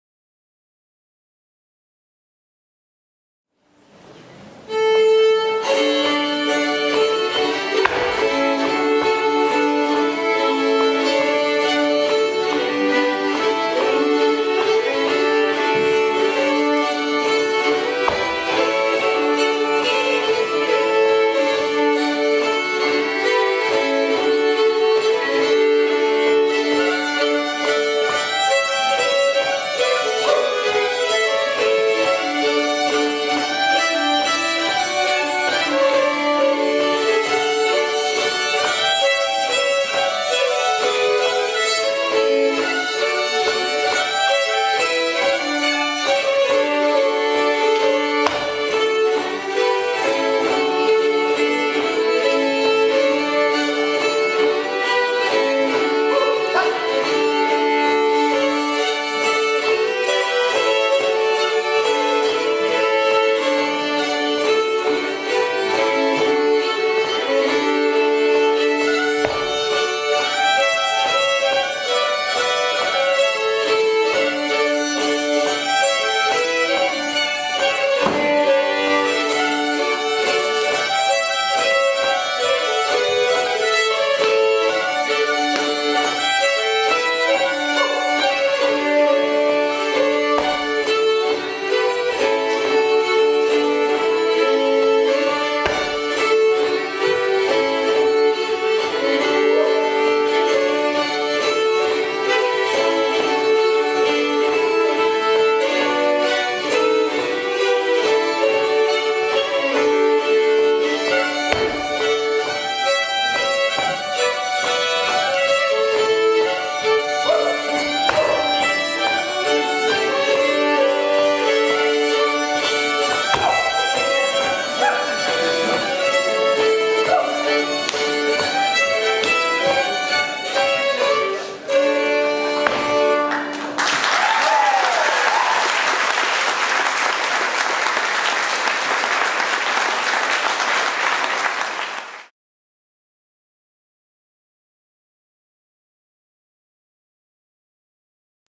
Østerdalspols - Solveig Brekke Hauknes og Ole Nilssen
Solveig Brekke Hauknes og Ole Nilssen danser østerdalspols i B-klassen på Landskappleiken 2013 på Røros.
Dorte Nilssen, Marit og Thomas Westling spiller.